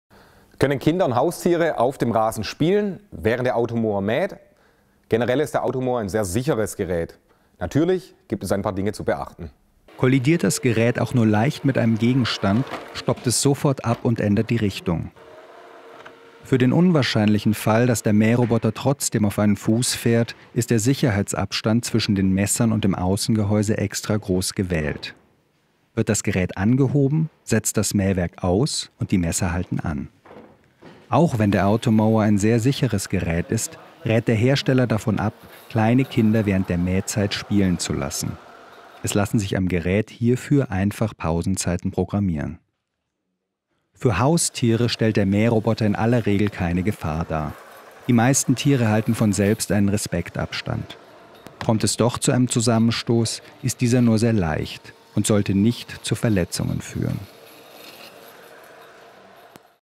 Sind Mähroboter eine Gefahr für Kinder und Hunde? | Contorion
Ganz schön bequem, so ein Mähroboter wie etwa der Automower von Husqvarna. Denn er hält den Rasen gleichmäßig kurz, indem er regelmäßig seine Bahnen zieht und das Gras auf Wunschlänge kürzt. Aber stellt ein Mähroboter mit dem unterseitigen Mähmesser eigentlich eine Verletzungsgefahr für spielende Kinder oder Haustiere auf dem Rasen dar? Generell ist der Automower von Husqvarna ein sehr sicheres Gerät, aber trotzdem gibt es das ein oder andere zu beachten. Was genau das ist, erklärt der Experte im Video.

Kollidiert der Mähroboter mit einem Gegenstand beziehungsweise mit einem Hund oder Kind, stoppt er zum Beispiel sofort. Für den unwahrscheinlichen Fall, dass der Automower auf einen Fuß fährt, ist der Sicherheitsabstand zwischen den Messern und dem Außengehäuse vom Hersteller extra groß gewählt. Wird das Gerät angehoben, halten die Messer außerdem sofort an. Trotzdem rät der Hersteller des Automower, kleine Kinder nicht zu Mähzeiten auf dem Rasen spielen zu lassen, und zeigt im Video die praktische Mähpausen-Funktion, die man am Gerät mit wenigen Knopfdrücken einstellen kann.